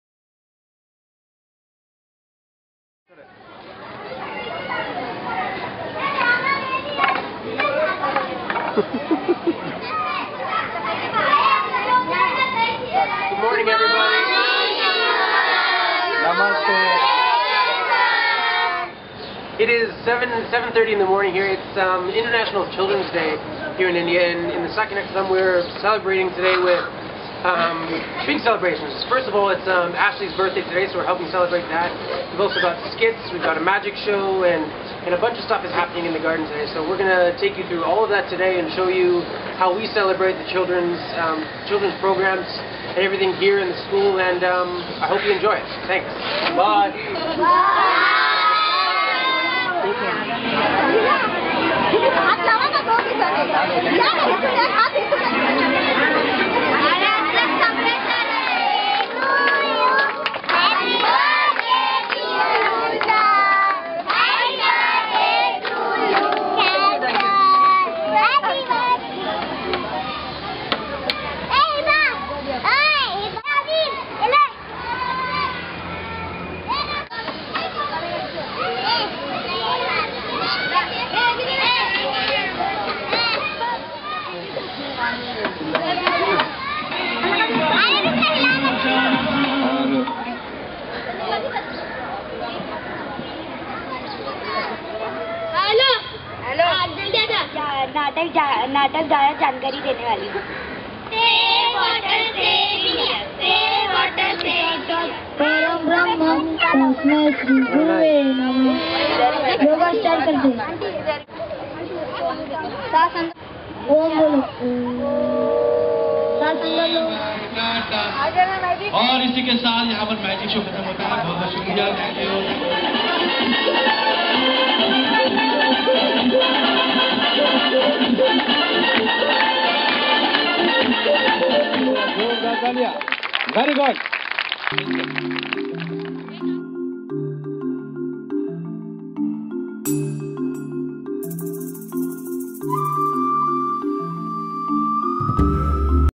Children's day,Dirty Wall Project
Children's day celebration , Saki Naka slum community.